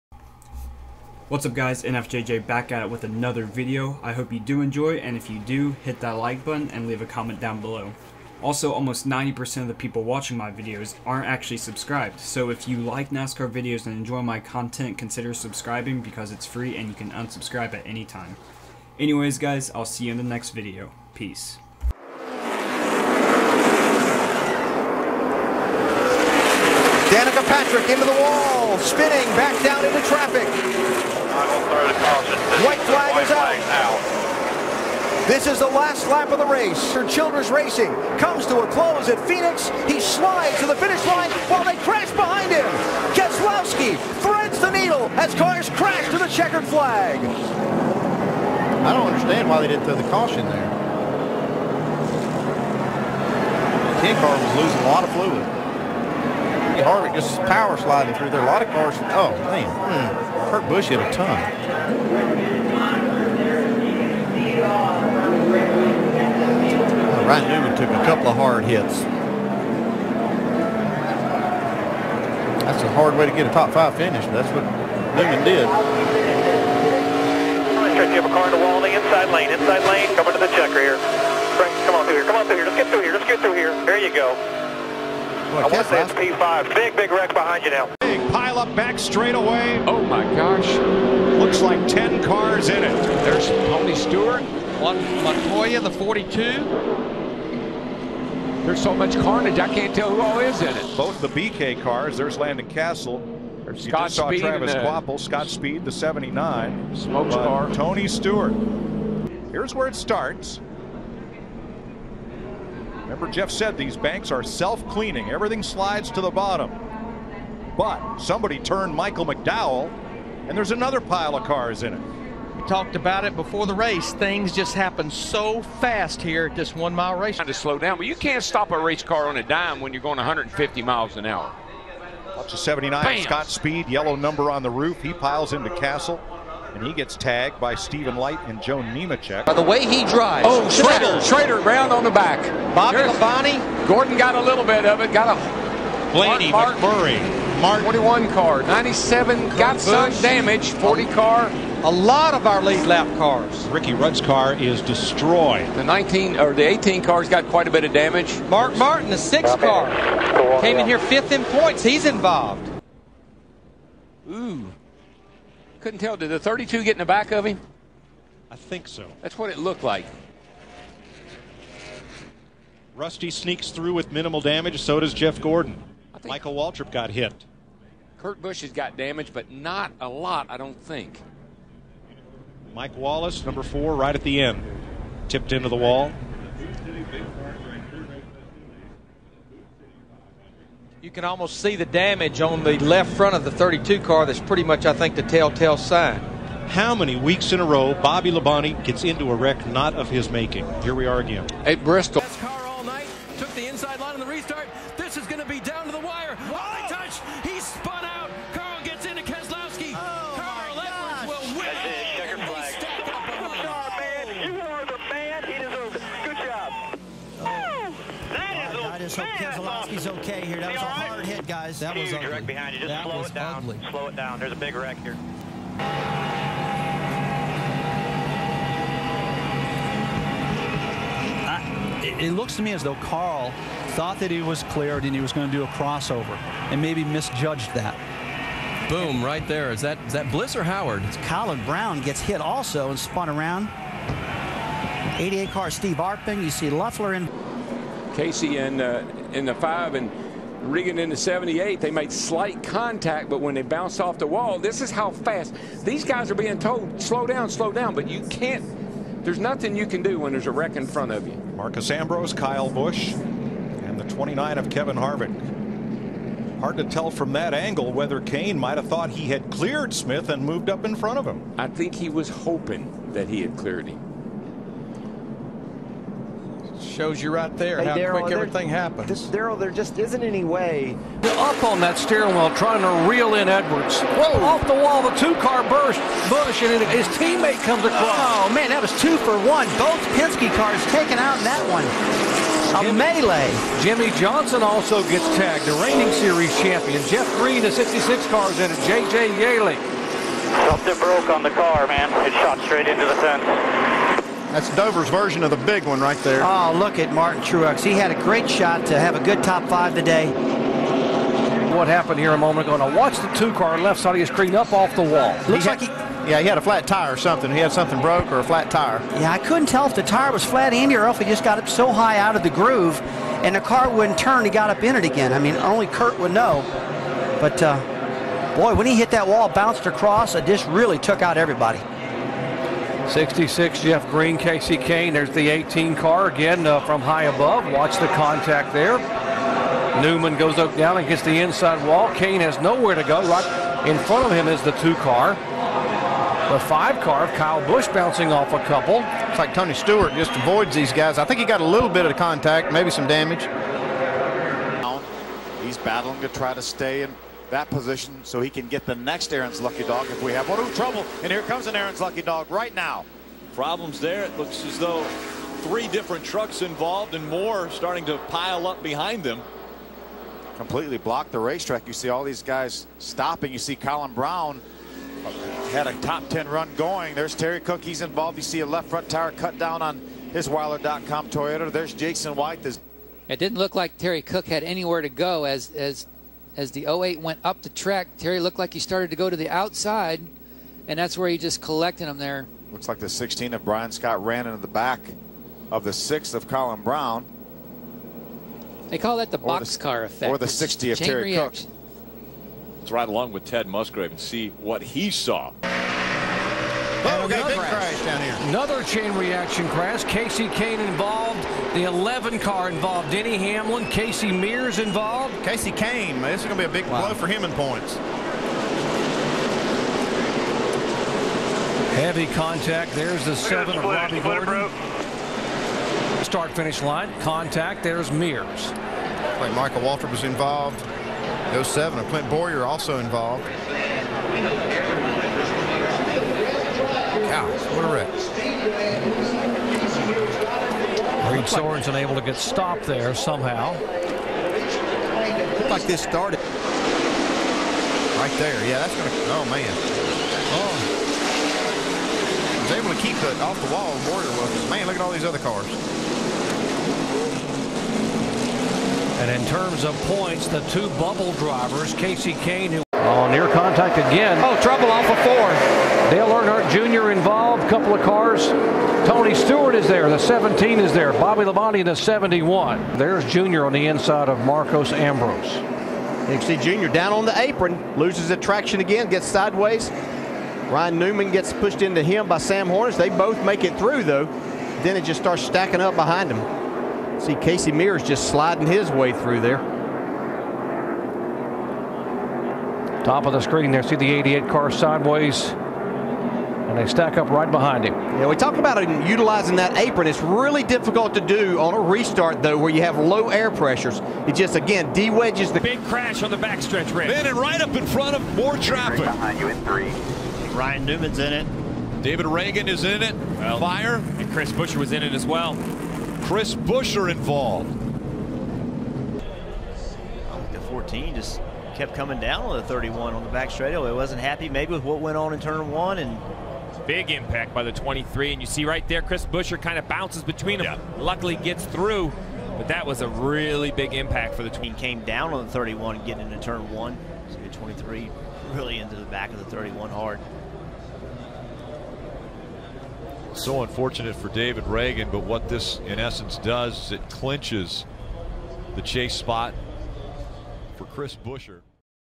NASCAR Short Track Big Ones
For the sake of the video, I considered tracks that have the "feel" of short tracks as well, essentially anything smaller than intermediate tracks. We really only have 3 proper short tracks, so that would be pretty stale to watch imo.

Part 2 will come out in a few weeks!

Also leave a comment letting me know your favorite part or how I can improve!